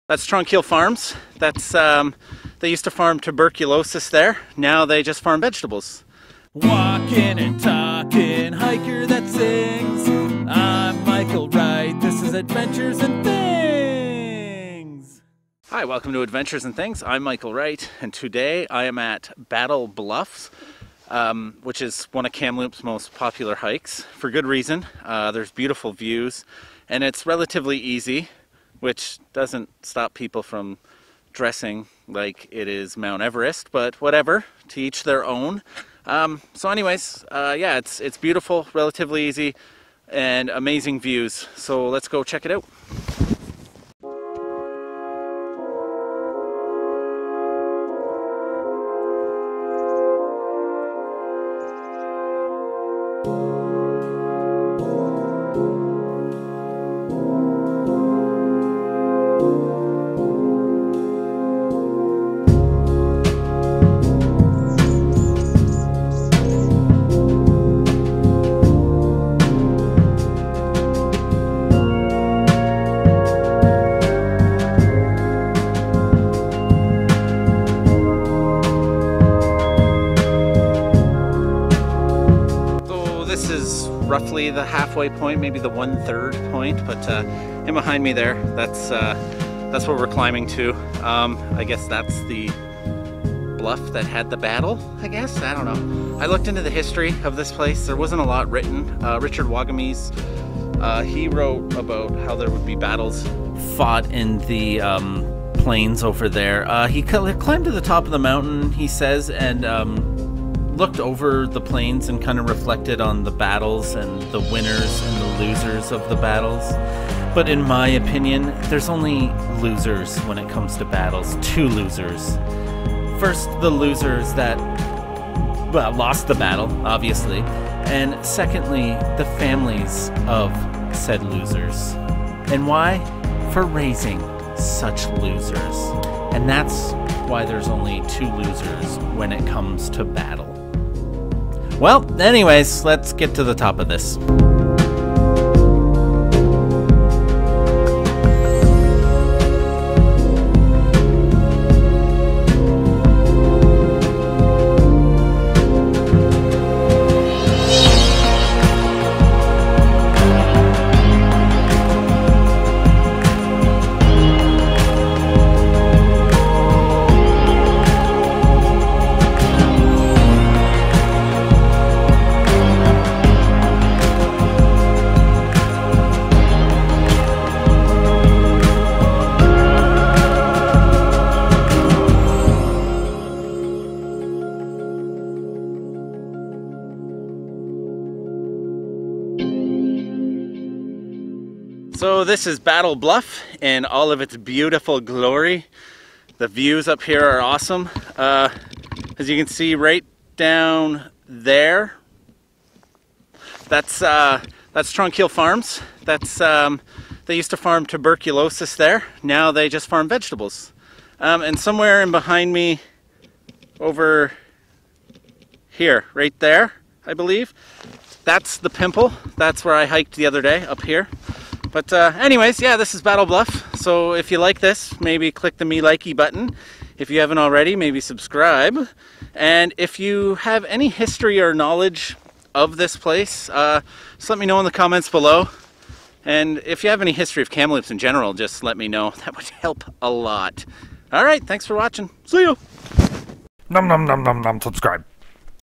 BATTLE BLUFF i t he winter. Kamlopps BC
This is my battle... bluff hike to one of Kamloops BC's most popular hikes and for good reason. It has amazing views and is a relatively easy hike relativity is all relative anyways. The stunning views of Kamloops if your vision is good enough, an overlooking view of Tranquille farms where they once farmed tuberculosis are what make the slight incline to the peak well worth the numbness of the left arm and the chest pains.